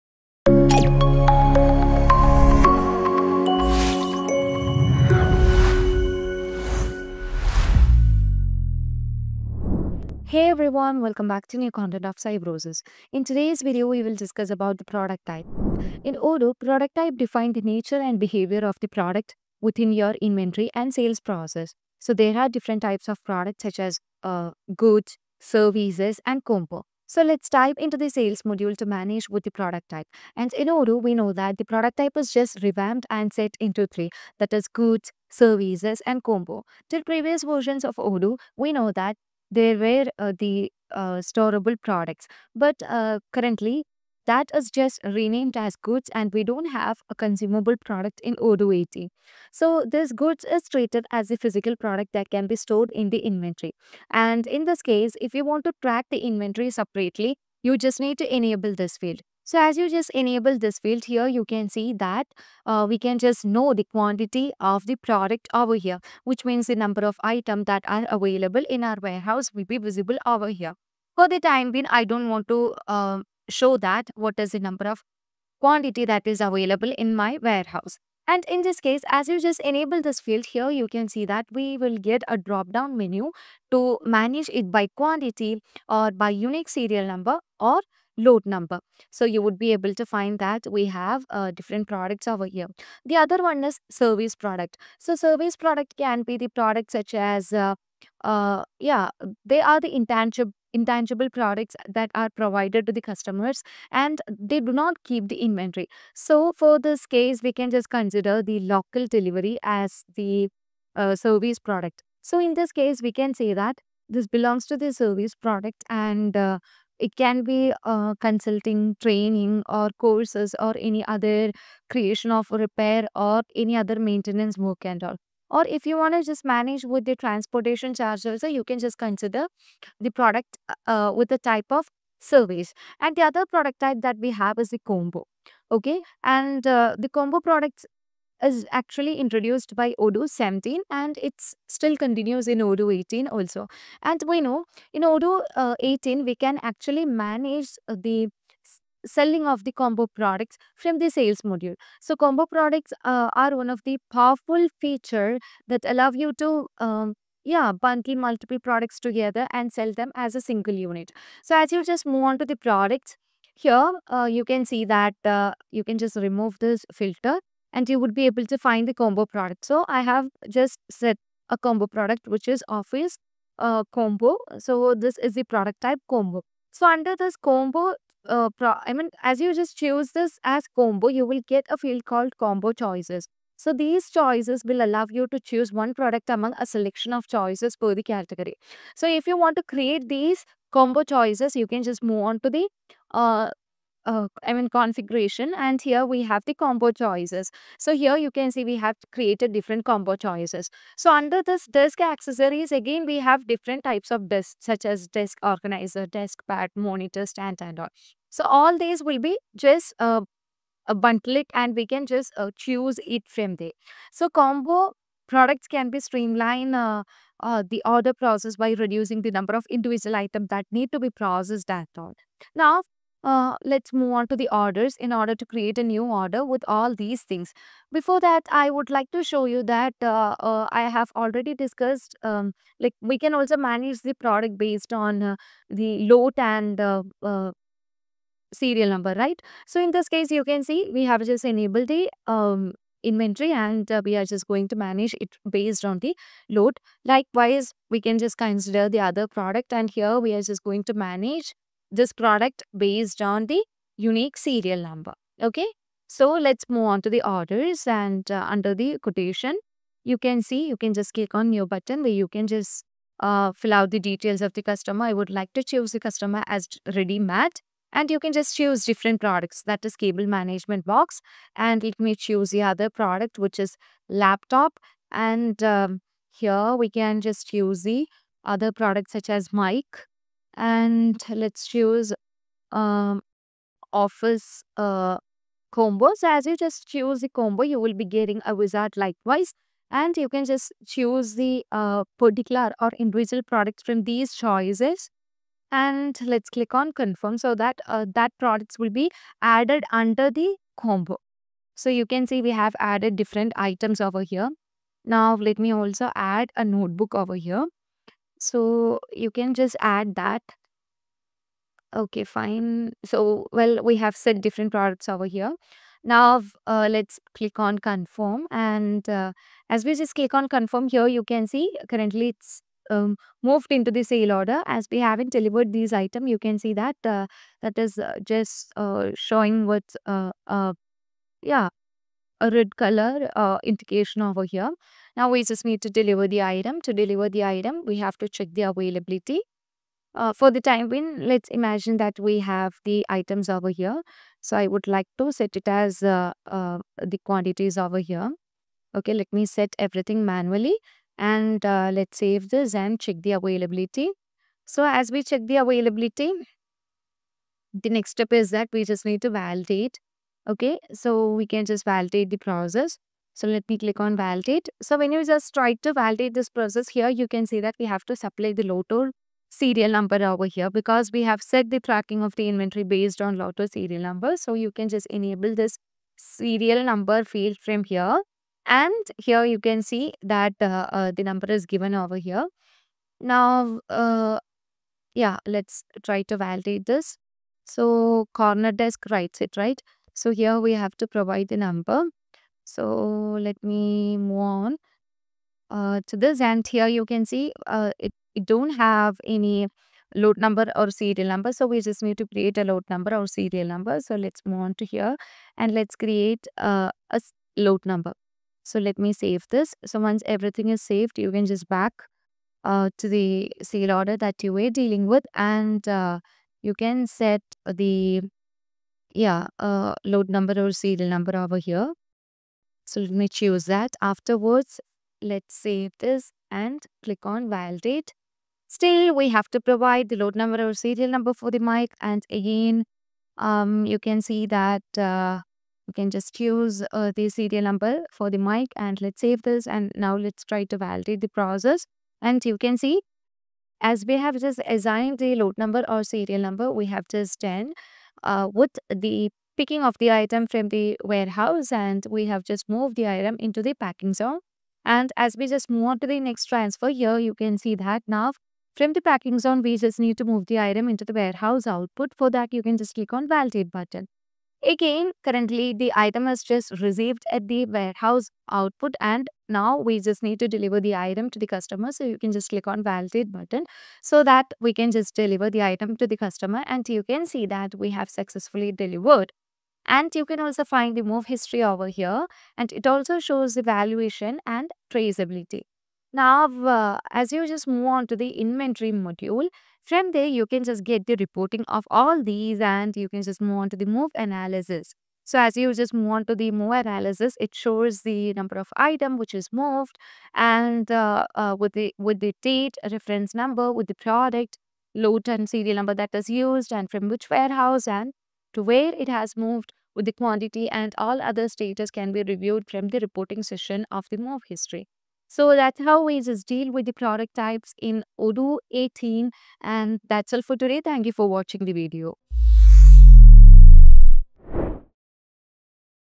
How to Manage Product Types in Odoo 18 Sales | Odoo 18 New Features | Odoo 18 Release
In Odoo 18, there are three main product types:

Goods: These are physical products that can be tracked in inventory. They can be further categorized as:
Storable Products: Products that are tracked in inventory and have a specific quantity on hand.
Consumable Products: Products that are consumed upon use and don't have a specific quantity on hand.
Services: These are intangible services that are not tracked in inventory.
Combo Products: These are products that are composed of multiple other products.

Key points to remember:

The distinction between storable and consumable products is now made by enabling or disabling the "Track Inventory" option for a product.
The "Consumable" and "Storable" product types from previous Odoo versions have been merged into the "Goods" type.
Combo products allow you to bundle multiple products together and sell them as a single unit.

How to manage product types in Odoo 18:

Go to Inventory -- Products.
Create a new product.
In the Type field, select the desired product type.
Configure the product's other details, such as name, description, cost price, selling price, etc.

Additional features in Odoo 18:

Product Attributes and Variants: You can create product attributes (like color, size, material) and variants (specific combinations of attributes) to manage product variations.
Product Categories: You can organize your products into categories to improve search and filtering capabilities.
Product Images: You can upload images for your products to enhance their visual appeal.
By understanding these product types and their configurations, you can effectively manage your inventory and sales in Odoo 18.